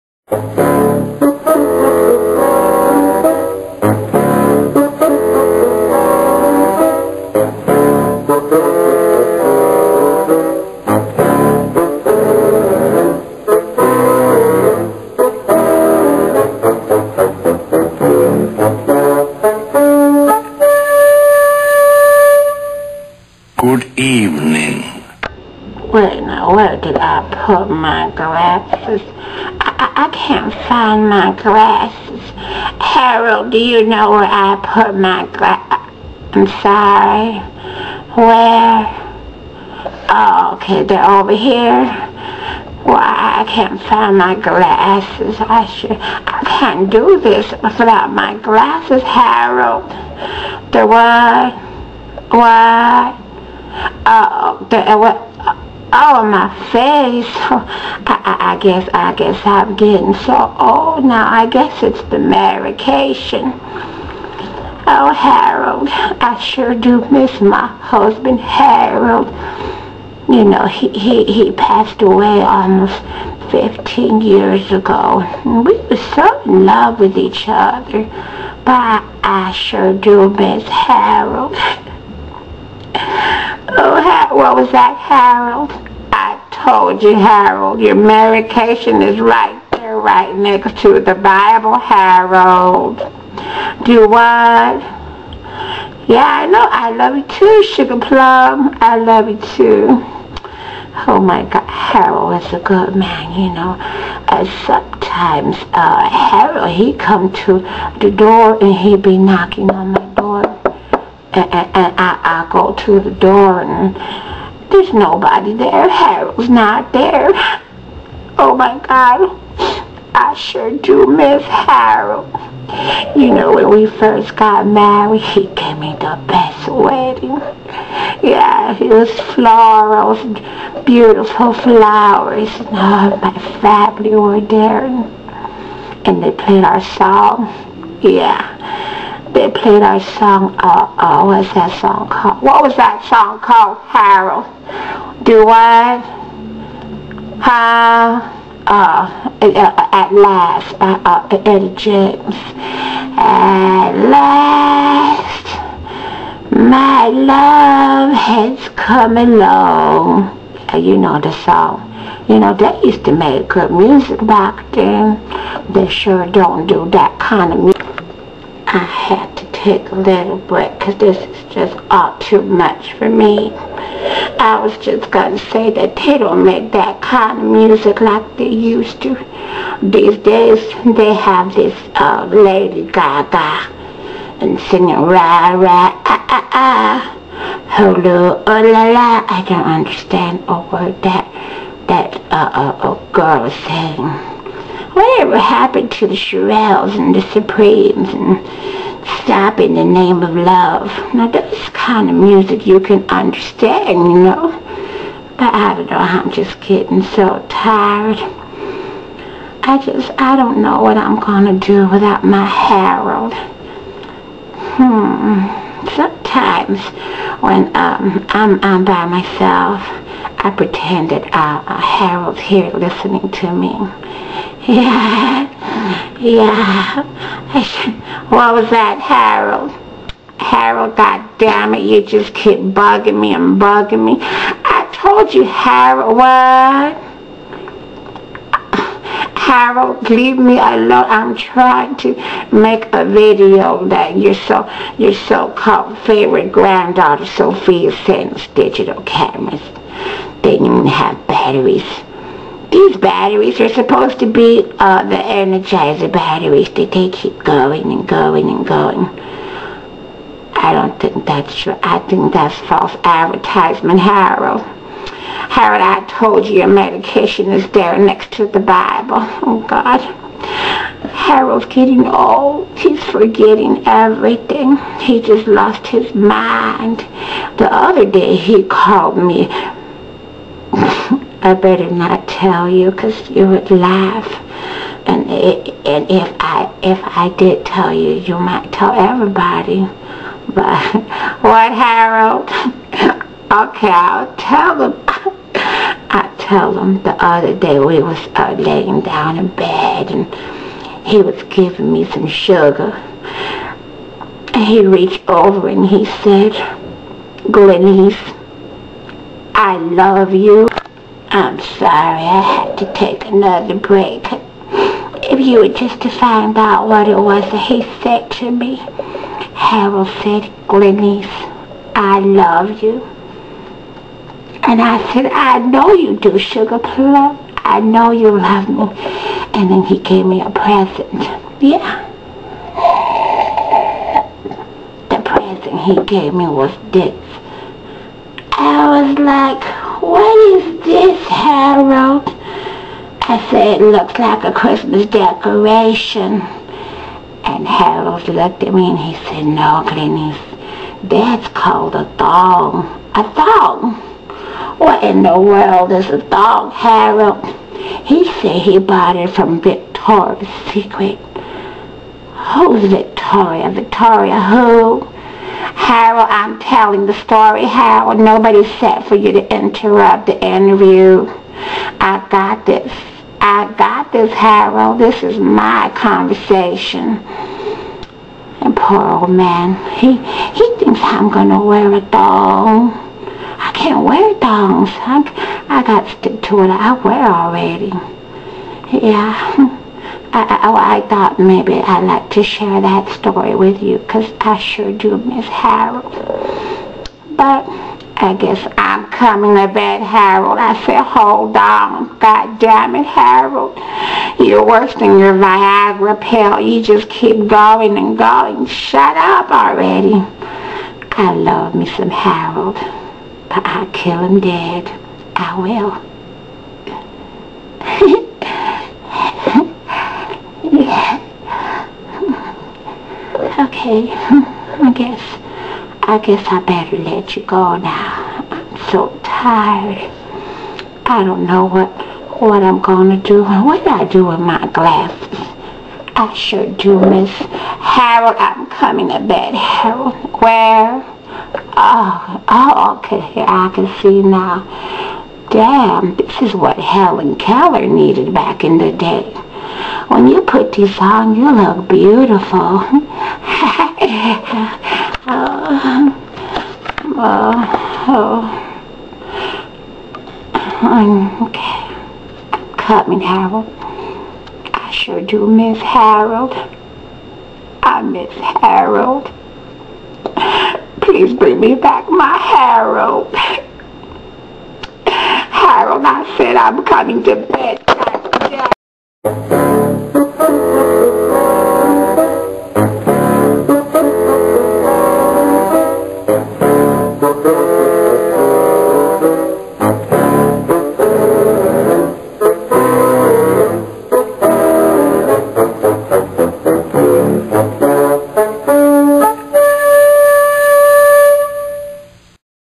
Did she or didn't she kill him?
Tracie is Glyniece, an old, supposedly senile woman, who imagines her husband is still alive 15 years after his death... and watch to the very end...the ending may suprise you